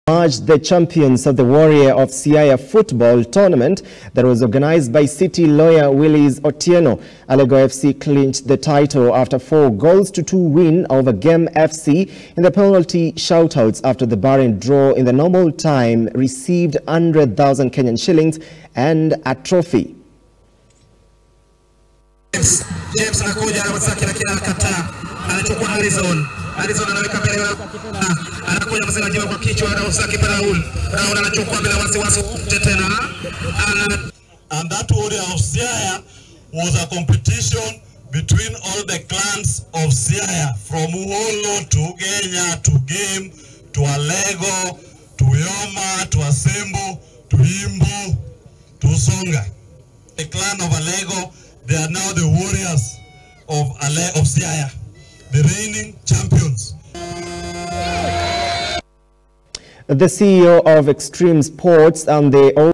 ALEGO FC GO HOME WITH KSH. 100,000 AND A TROPHY
Siaya Football Competition
Alego Fc Emerged The Champions Of The Warrior Of Siaya Football Tournament That Was Organized By City Lawyer Willis Otieno.Alego Fc Clinched The Title After Four Goals To Two Win Over Gem Fc In The Penalty Shoutouts After A Barren Draw In The Normal Time And Received 100,000 Shillings And A Trophy.